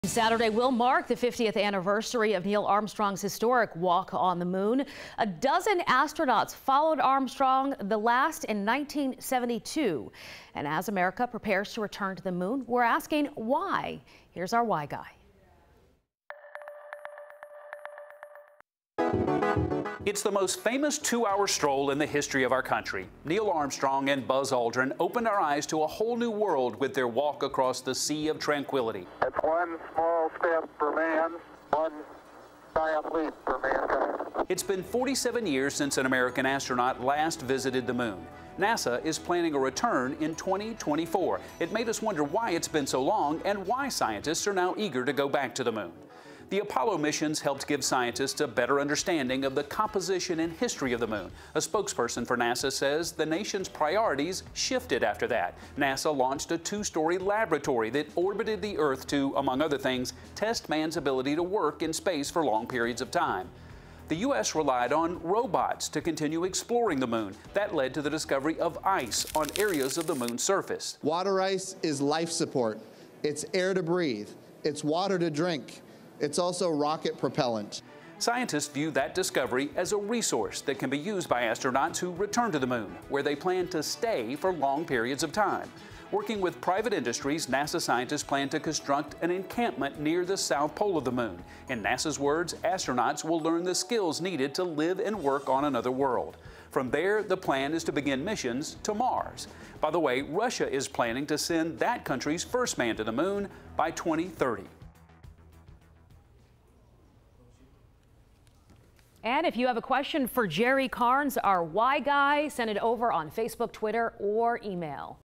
Why has it been so long since we went to the moon? And why are we going back?
The short of it is it's a matter of how space exploration and research priorities shifted after the Apollo missions. More news on 11Alive.com